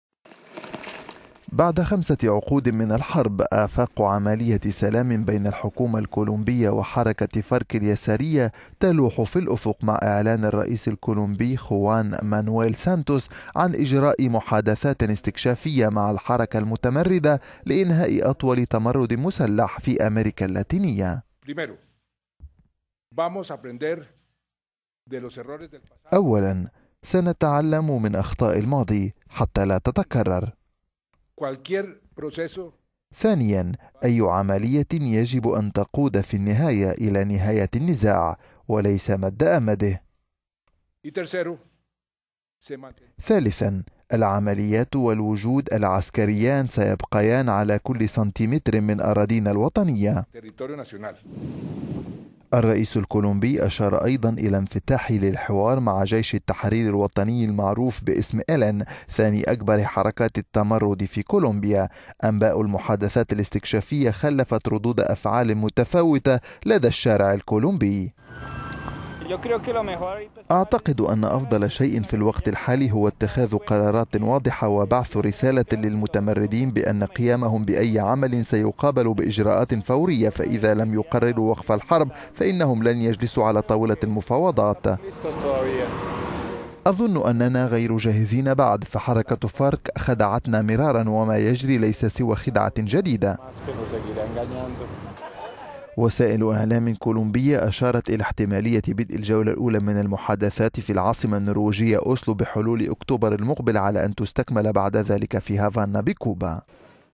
كولومبيا: إجراء محادثات سلام استكشافية بين الحكومة...
بعد خمسة عقود من الحرب، افاق عملية سلام بين الحكومة الكولومبية وحركة "فارك" اليسارية، تلوح في الافق مع اعلان الرئيس الكولومبي خوان مانويل سانتوس عن اجراء محادثات استكشافية مع الحركة المتمردة لانهاء اطول تمرد مسلح في امريكا اللاتينية.

 الرئيس الكولومبي خوان مانويل سانتوس يقول:
"أولا سنتعلم من اخطاء الماضي حتى لا تتكرر. ثانيا اي عملية يجب أن تقود في النهاية إلى نهاية النزاع وليس مد أمده. ثالثا العمليات والوجود العسكريان سيبقيان على كل سنتيمتر من اراضينا الوطنية"

الرئيس الكولومبي اشار ايضا إلى انفتاحه للحوار مع جيش التحرير الوطني المعروف باسم "إلن"، ثاني أكبر حركات التمرد في كولومبيا.

انباء المحادثات الاستكشافية خلفت ردود افعال متفاوتة لدى الشارع الكولومبي

وليام غوميز مواطن كولومبي يقول:
"اعتقد ان افضل شيء في الوقت الحالي هو اتخاذ قرارات واضحة وبعث رسالة للمتمردين بان قيامهم باي عمل سيقابل باجراءات فورية. فإذا لم يقرروا وقف الحرب فانهم لن يجلسوا على طاولة المفاوضات"

هكتور ريز مواطن كولومبي يقول:
"اظن اننا غير جاهزين بعد، فحركة "فارك" خدعتنا مرارا وما يجري ليس سوى خدعة جديدة"

وسائل اعلام كولومبية اشارت إلى احتمالية بدء الجولة الاولى من المحادثات في العاصمة النرويجية اوسلو بحلول اكتوبر المقبل على ان تستكمل بعد ذلك في هافانا.